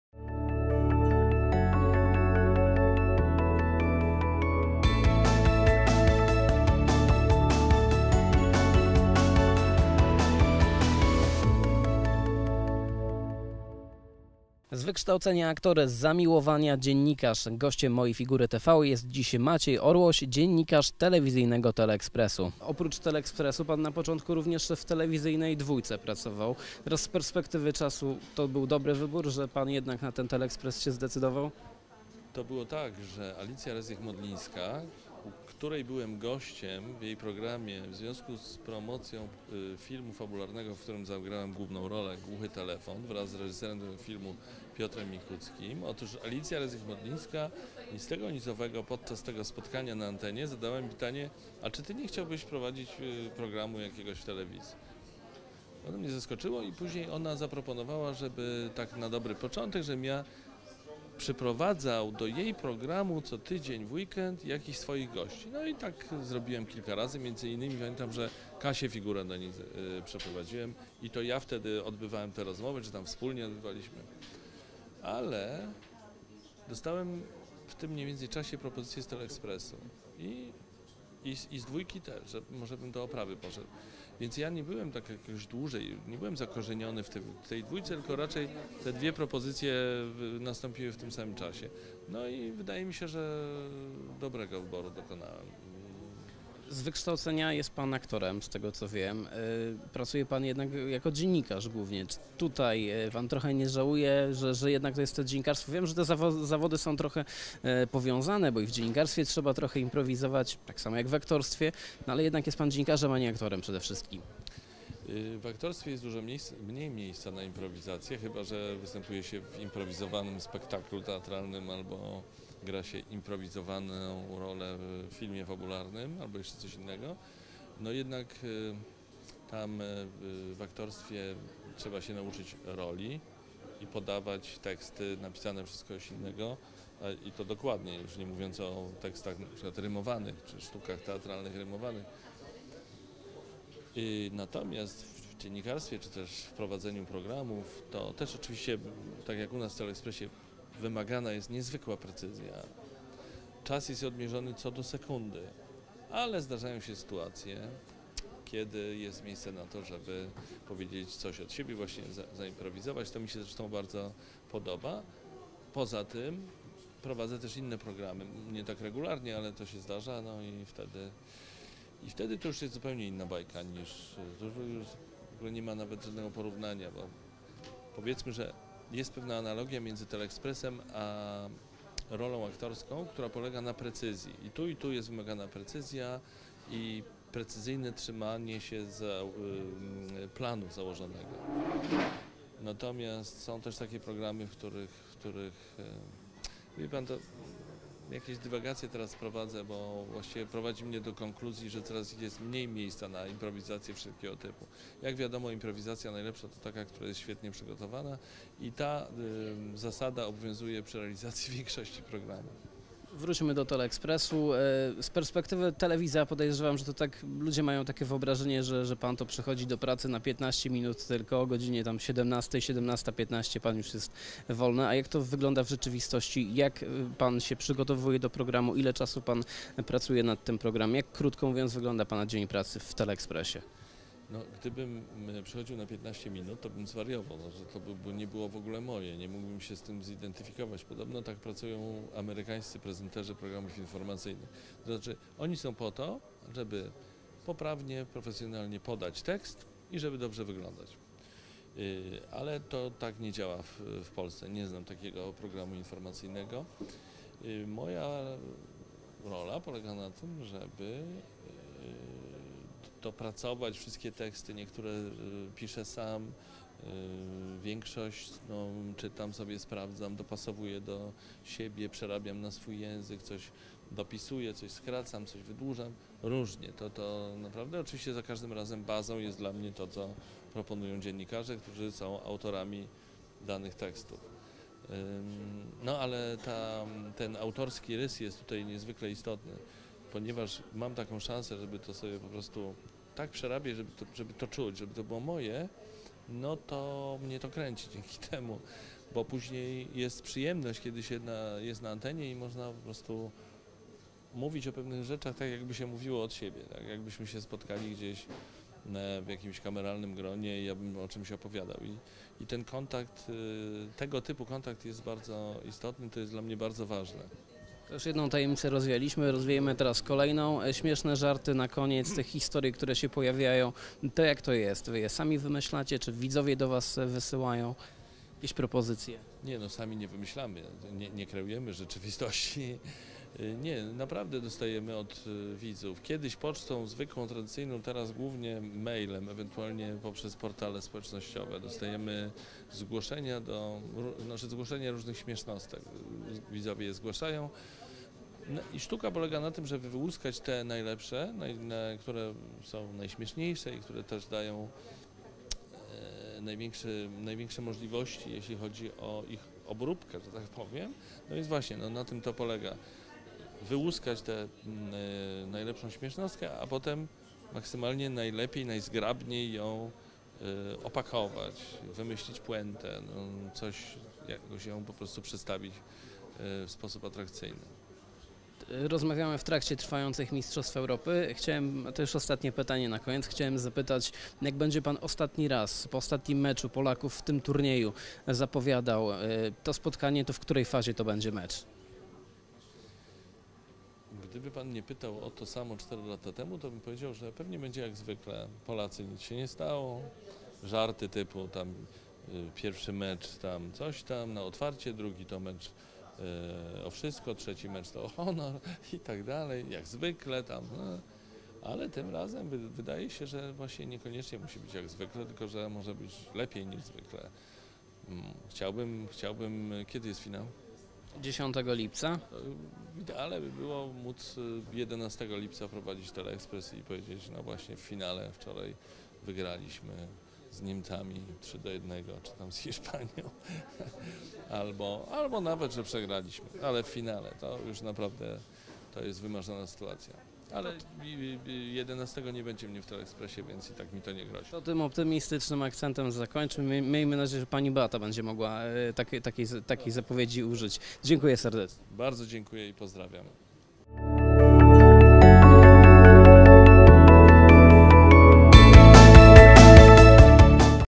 Maciej Orłoś - Jak trafiłem do Teleexpresu
Znany dziennikarz wyjaśnia w jaki sposób trafił do telewizji prosto z radia. Zdradza nam również kulisy powstawania codziennych programów Teleexpressu oraz kilka ciekawostek związanych z tym popularnym programem informacyjnym, których nie znają telewidzowie.